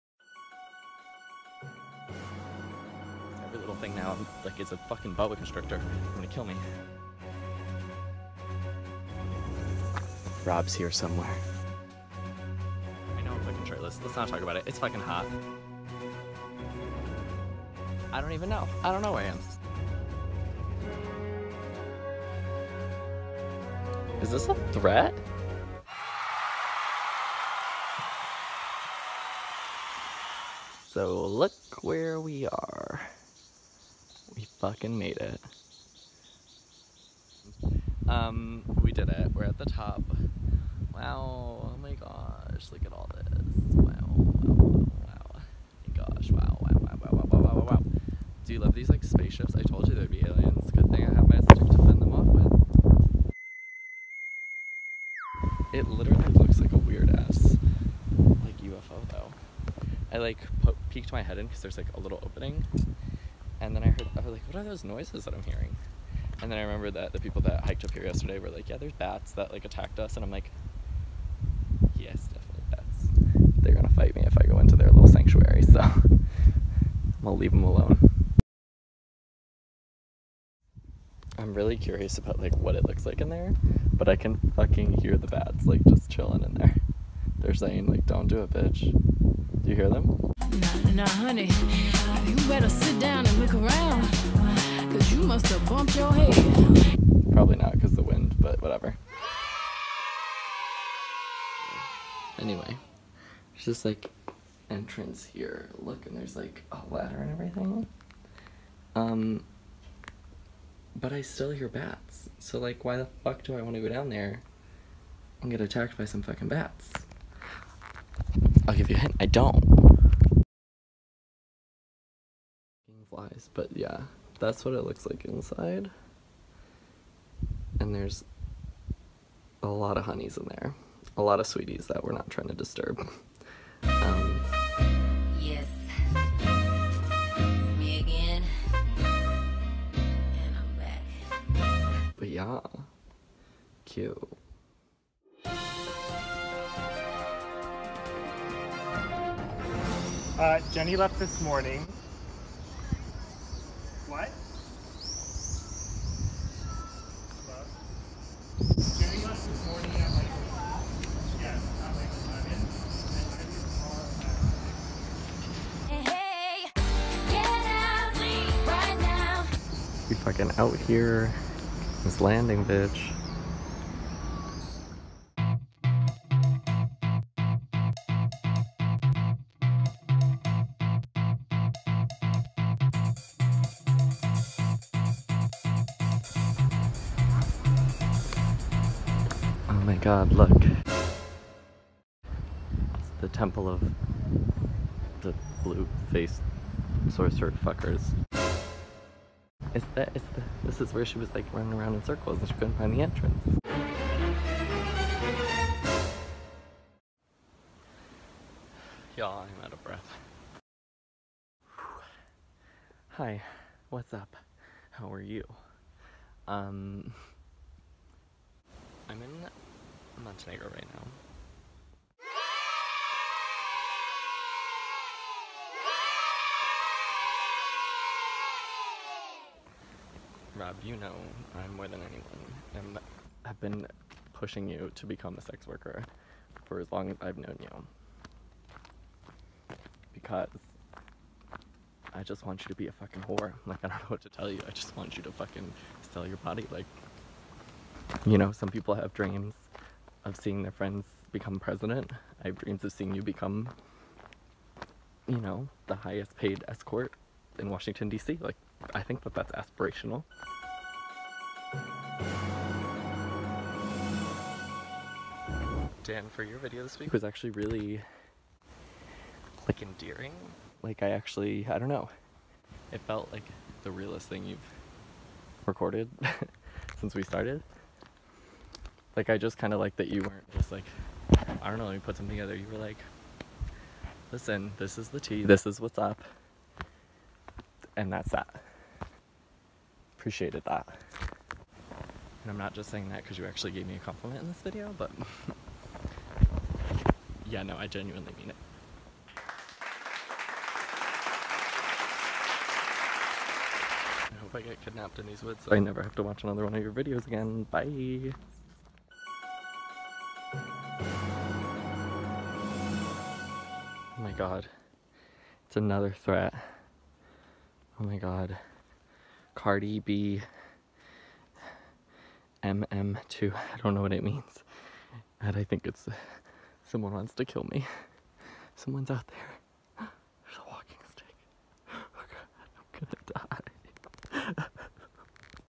IS BAT BOY GAY?!
I complete my transformation into a cave creature and actually don’t insult Dan and Rob for once.

Sorry the audio is fucked but not really.